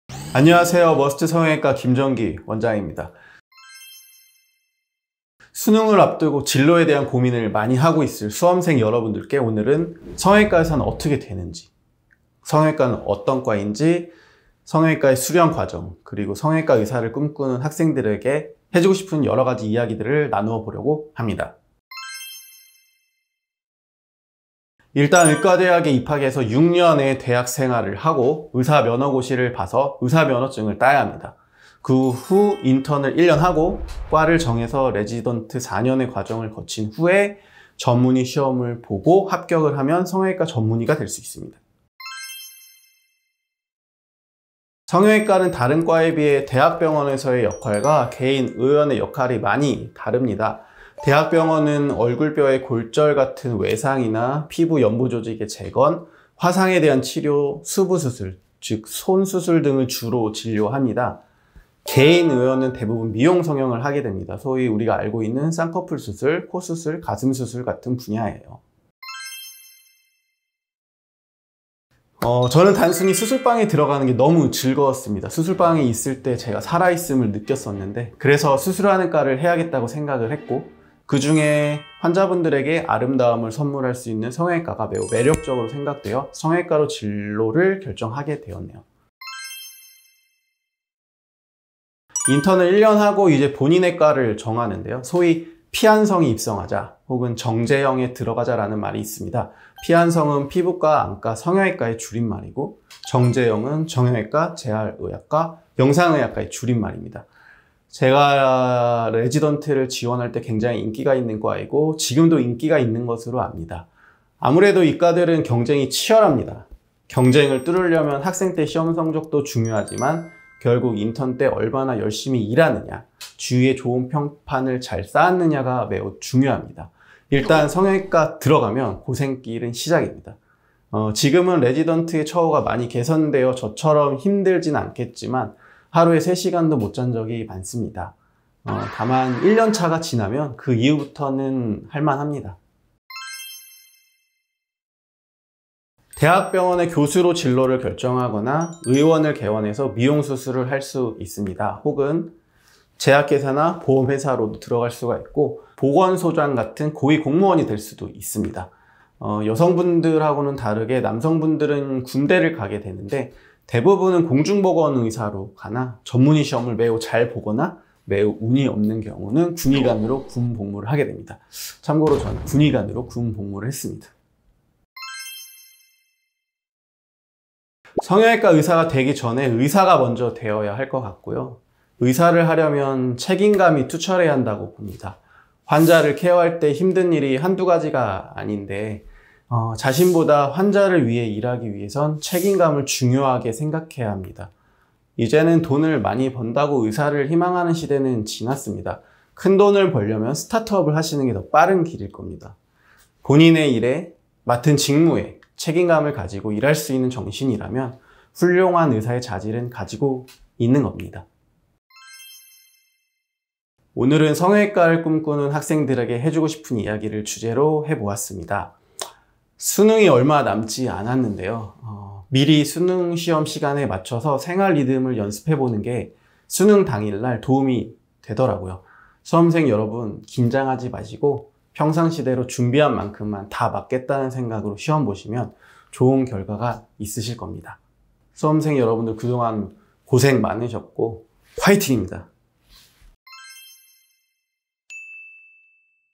성형외과 의사가 되는법
안녕하세요. 머스트 성형외과입니다.
11월17일에 있을 중요한 시험이 있죠?
바로 수능시험인데요
오늘은 성형외과 의사가 되는 방법을 말씀해드리겠습니다
머스트 성형외과는 수험생 여러분을 응원하겠습니다!

00:00 인트로
00:22 성형외과 의사는 어떻게 되나요?
00:41 성형외과는 어떤 과인가요?
01:10 성형외과를 선택한 이유?
01:30 성형외과의 수련 과정
02:22 성형외과 전문의가 된 후 진로
02:55 성형외과를 꿈꾸는 학생들에게 마지막 한마디